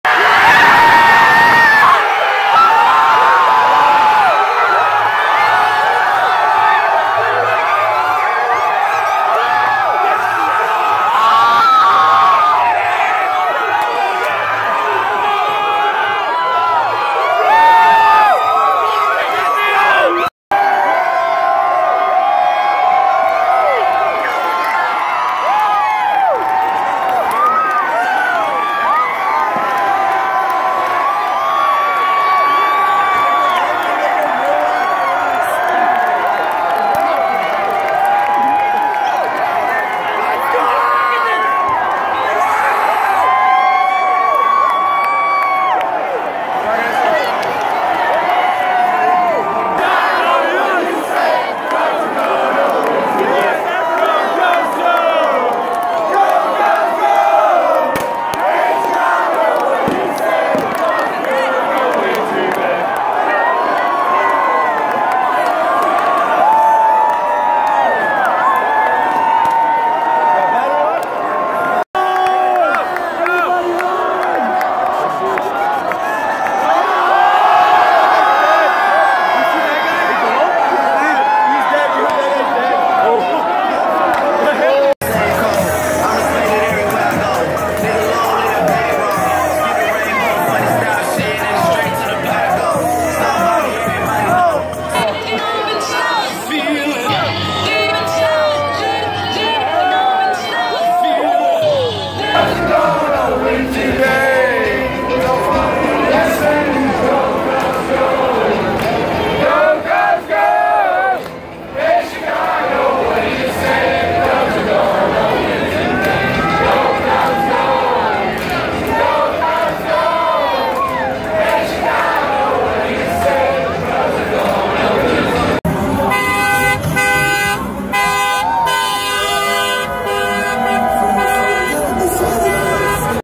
Cubs Win! Wrigley Celebration
Cubs WIN World Series 2016!
Clips from all around Wrigley and how crazy people got!